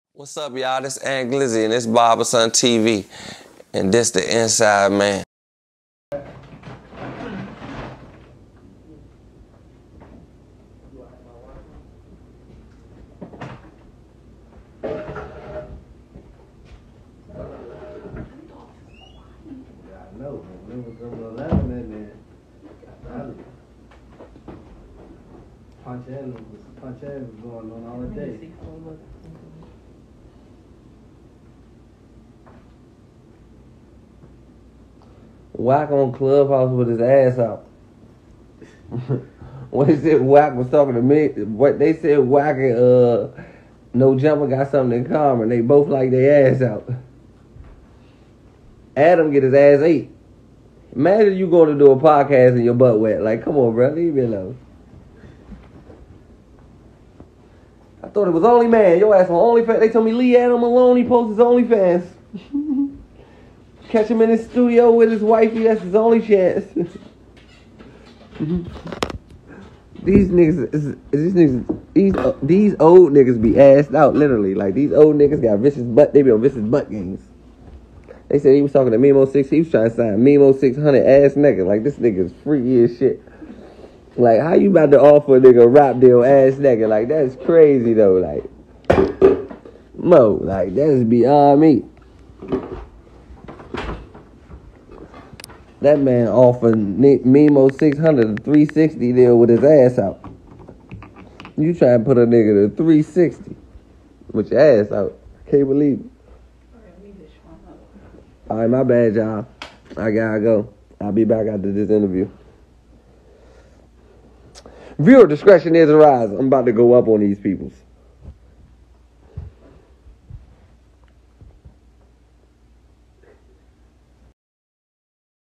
Ant Glizzy - Wack 100 had his A$S Out while talking to Memo600.. Leave me alone..
I just replay Ant Glizzy Instagram Live's...